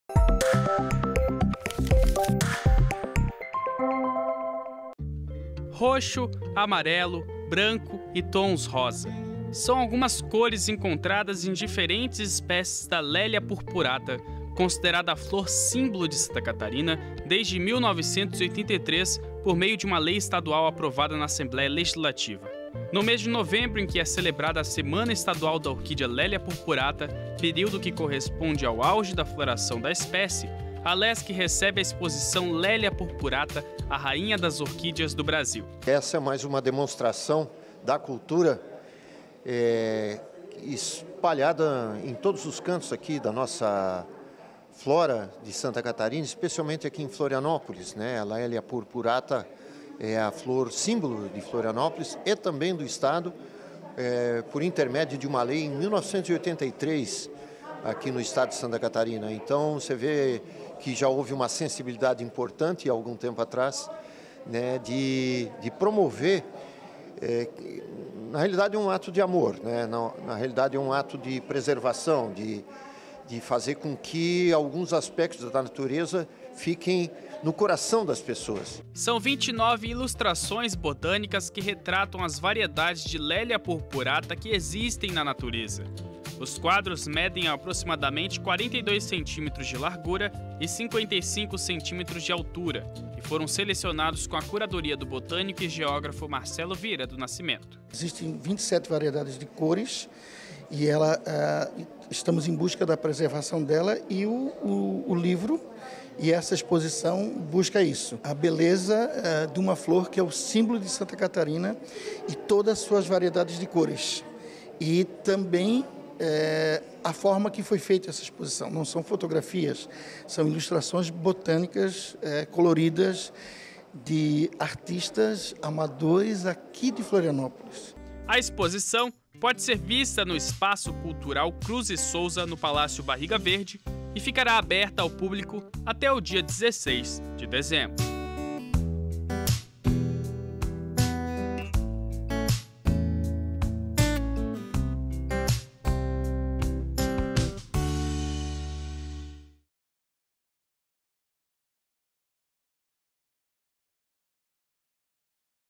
É Notícia - Alesc recebe exposição “Laelia Purpurata, a rainha das orquídeas do Brasil” - 22/11/22
A exposição “Laelia Purpurata, a rainha das orquídeas do Brasil” está aberta à visitação no espaço cultural Cruz e Sousa, no Palácio Barriga Verde, em Florianópolis. As obras ficarão expostas até o dia 16 de dezembro.